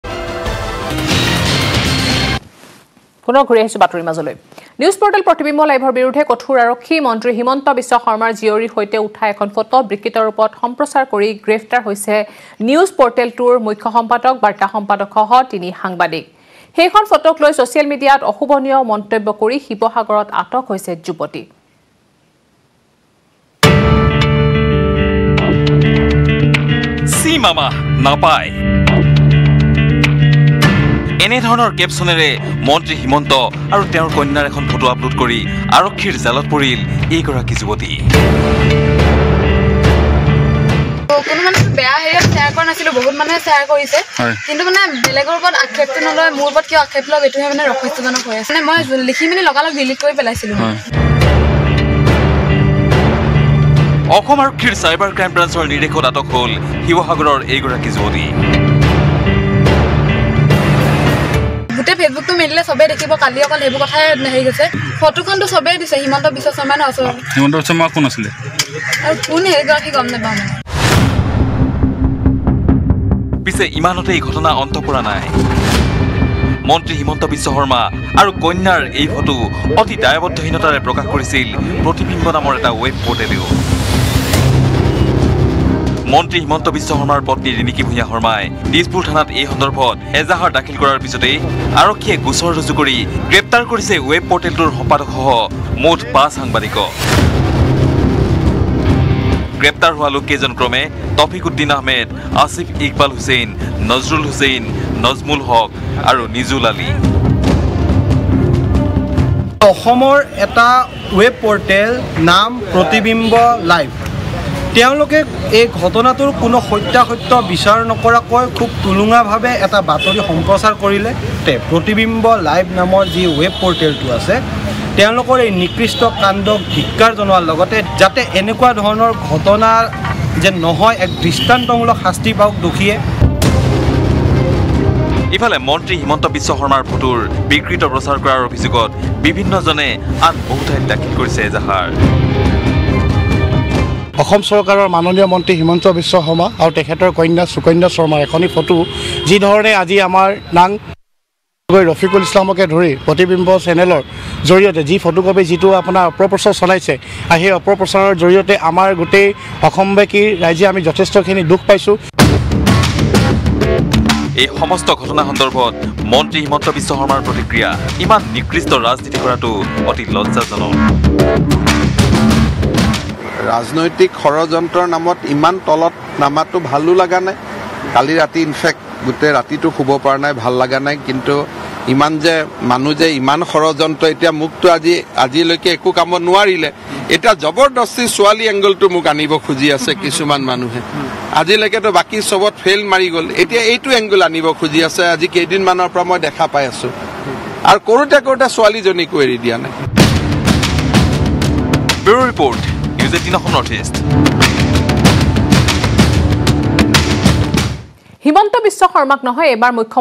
হিমন্তবিশ্ব শৰ্মাৰ জীয়ৰিৰ বিকৃত ফটো আপলোড কৰি বিপদত Sivasagarৰ যুৱতী
প্ৰতিবিম্ব লাইভ নামৰ ৱেব পৰ্টেলৰ মালিক তফিকউদ্দিন আহমেদ আৰু News Editor এজনক গ্ৰেপ্তাৰ সন্দৰ্ভত হিমন্তবিশ্ব শৰ্মাৰ প্ৰতিক্ৰিয়া । হিমন্তবিশ্ব শৰ্মাৰ ছোৱালী বিকৃত কৰাৰ অভিযোগ উত্থাপন কৰিছিল হিমন্তবিশ্ব শৰ্মাৰ পত্নী ৰিণিকি ভূঞা শৰ্মাই । News18 Assam Northeastত চাওঁক গুৰুত্বপূৰ্ণ সংবাদসমূহ।